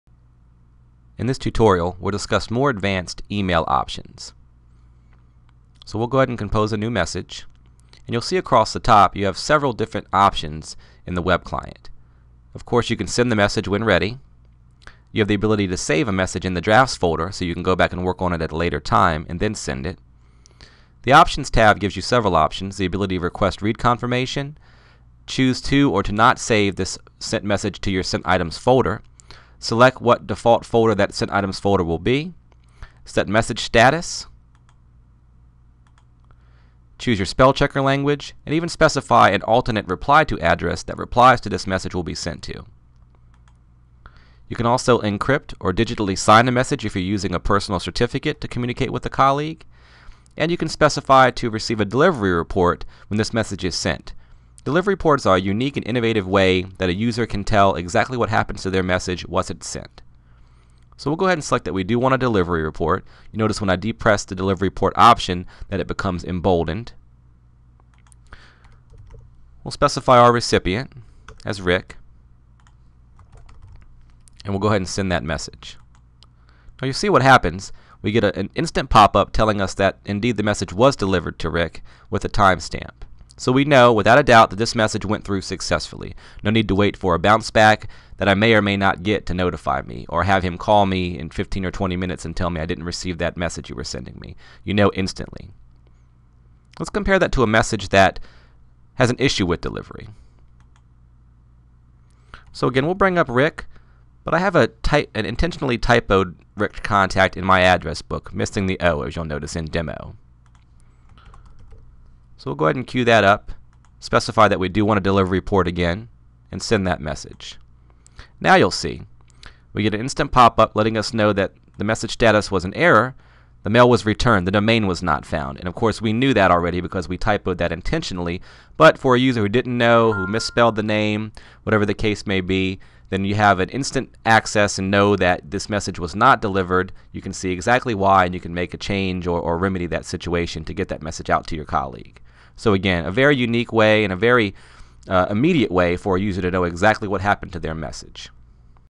IceWarp - Advanced Email Options in the IceWarp WebClient
Learn about many of the advanced options when composing emails in the IceWarp WebClient.